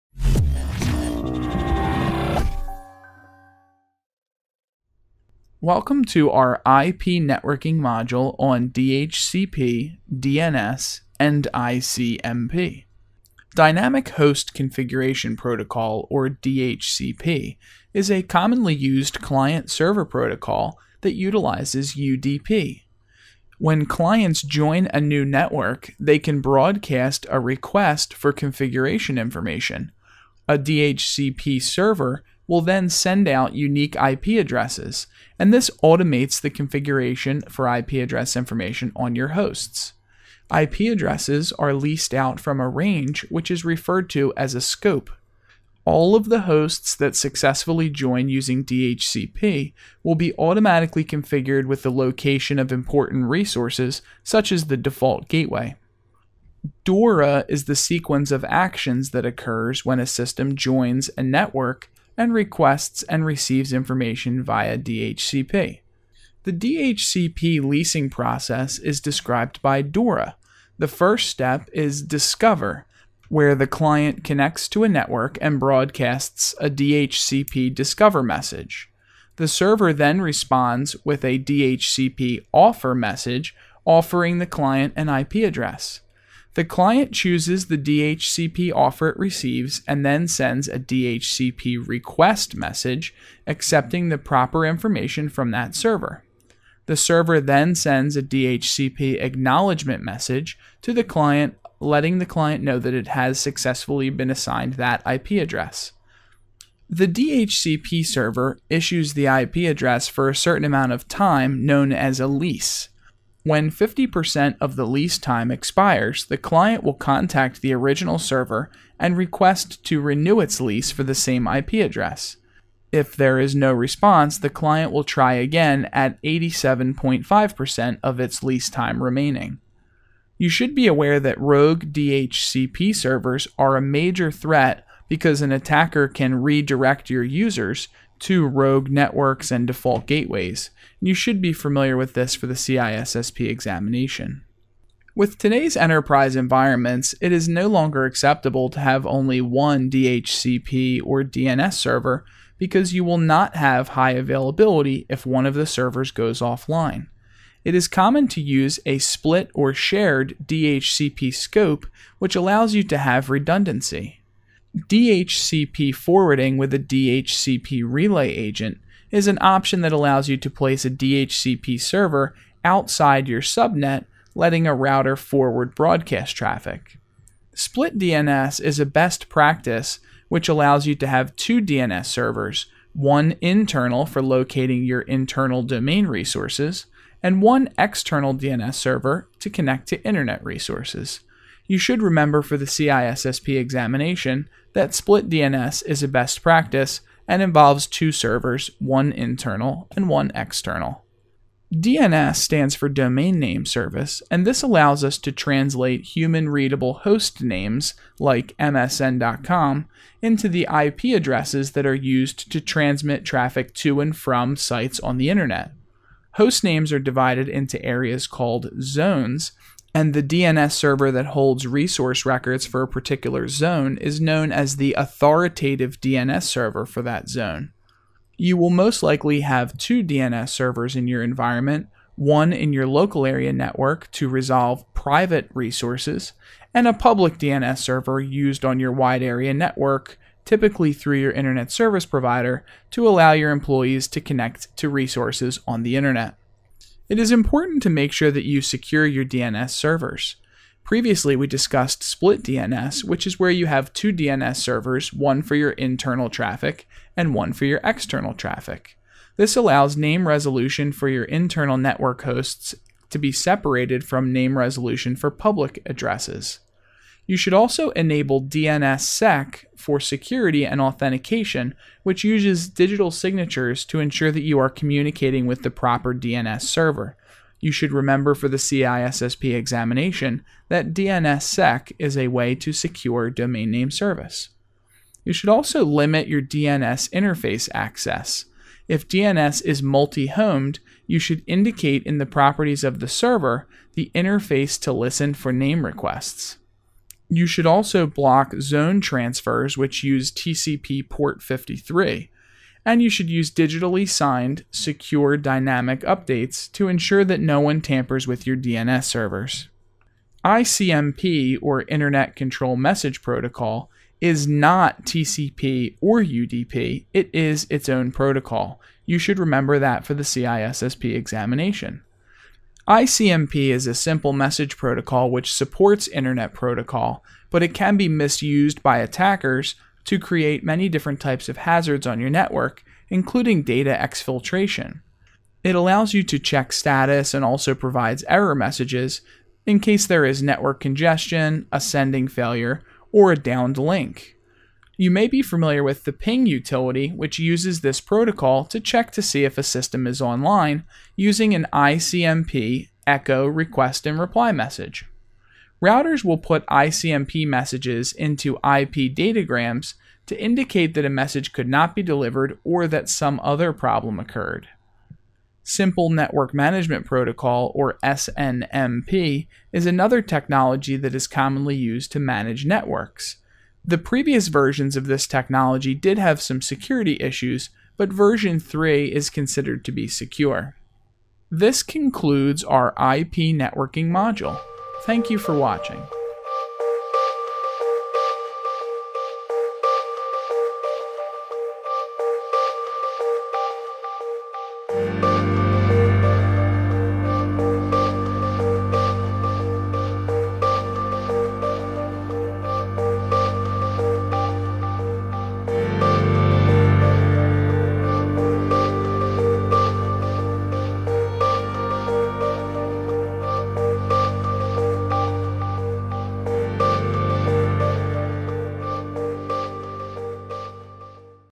DNS and DHCP (CISSP Free by Skillset.com)
This DNS and DHCP training video is part of the CISSP FREE training course from Skillset.com

Topic: DNS and DHCP
Skill: Network Security Fundamentals
Skillset: Communication and Network Security
Certification: CISSP